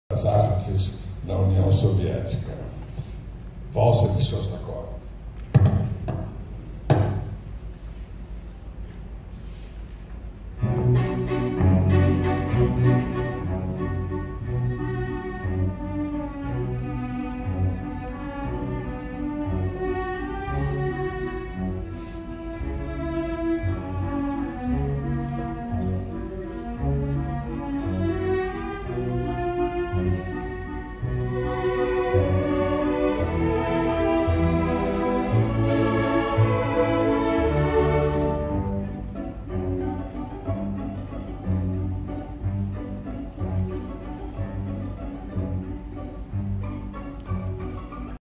Tilibra 90 anos - Maestro João Carlos Martins [3/8]
A Bachiana Filarmônica SESI-SP, sob regência do maestro João Carlos Martins, se apresentou no dia 08 de dezembro, data oficial do aniversário de 90 anos da Tilibra, no Teatro Municipal de Bauru, em um evento gratuito e aberto para o público da cidade.